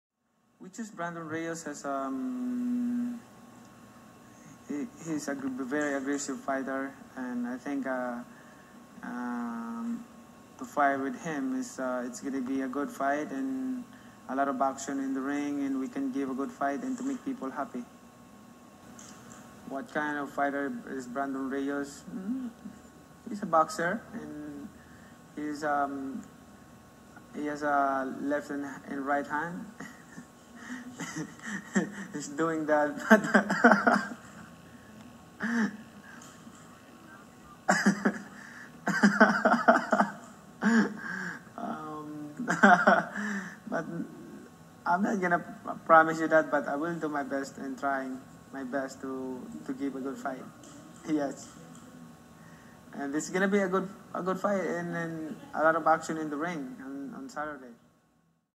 MANNY PACQUIAO CRACKS UP WHILE DISCUSSING TYPE OF FIGHTER BRANDON RIOS IS
"What kind of fighter is Brandon Rios? He's a boxer. He has a left and right hand," stated multi-division world champion Manny Pacquiao, who was all laughs when asked to describe the type of fighter he'll be facing when he steps into the ring with former lightweight champion Brandon Rios this Saturday. Check it out!